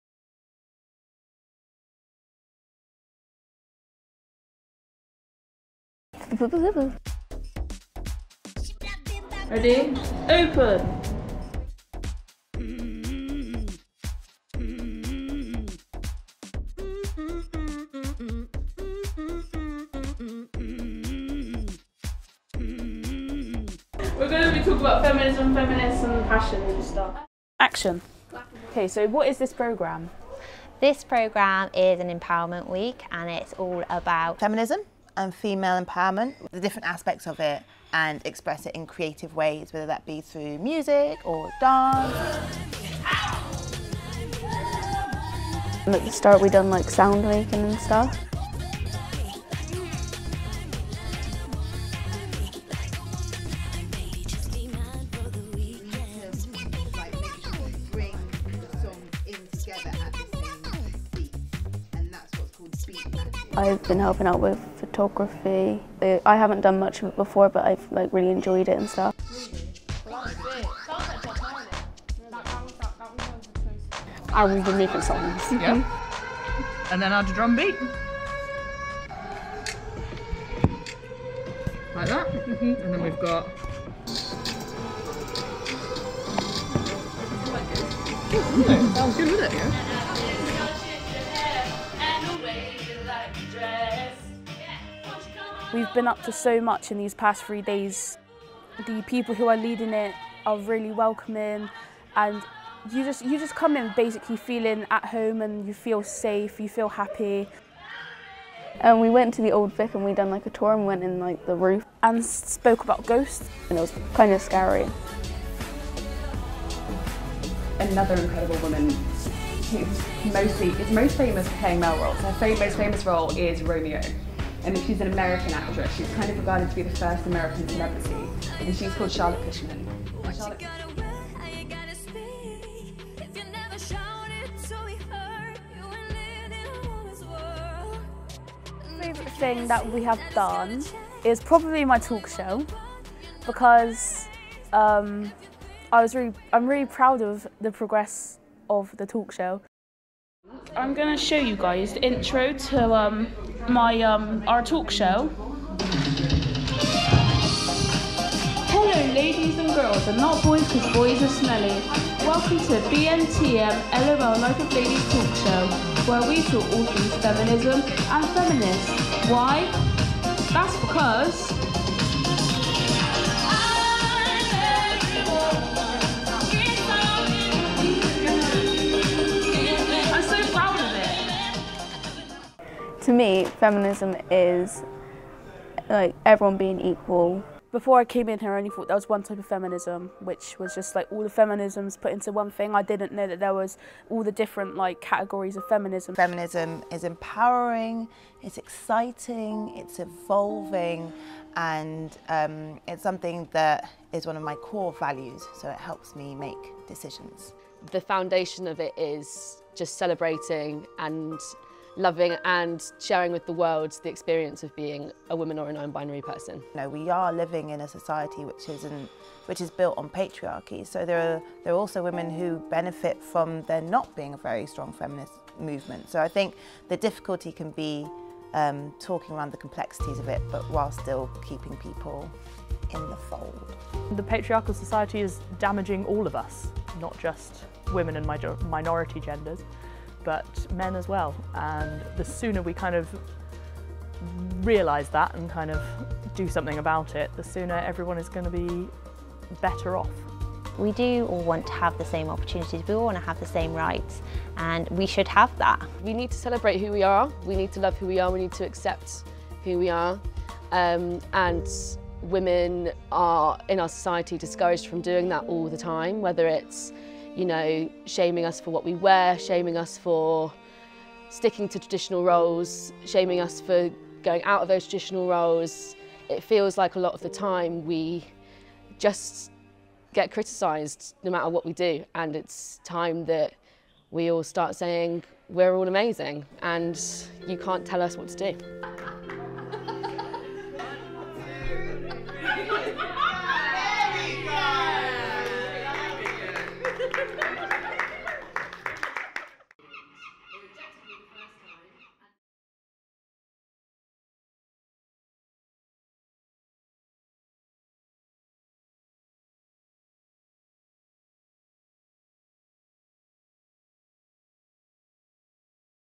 Empowerment: A film by Bristol young women and non-binary people
A film produced by the Bristol Beacon Empowerment Programme participants.

Our Empowerment programme directly empowers, builds confidence, and raises the voice of young women and non-binary young people.

The programme of activities co-created by the participants, explores themes of identity and feminism through activities such as music making, movement, public speaking and visits to cultural organisations. It aims to make a long-lasting positive difference, encouraging participants to engage further with cultural activity across the city and gain a greater sense of agency and wellbeing.

Empowerment is funded by the Paul Hamlyn Foundation and National Lottery Heritage.

Film produced by the participants in collaboration with Great Scott films.